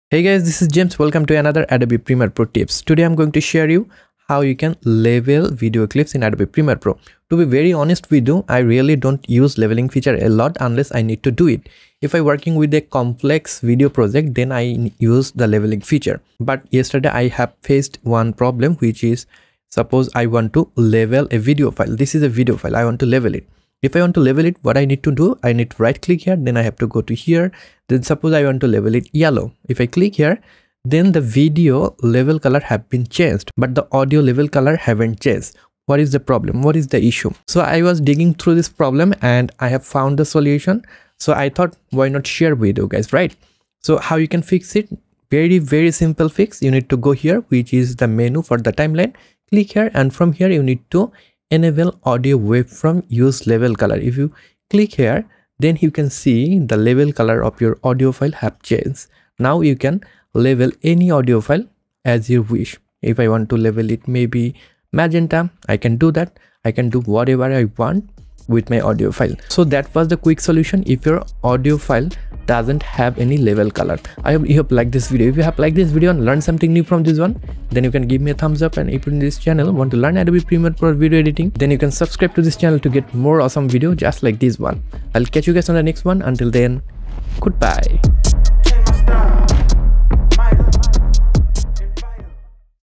Premiere Pro Audio Label Color is not Showing | Issue Fixed
Today I'll show you if your audio label color is not visible in Premiere Pro and how you can solve that issue.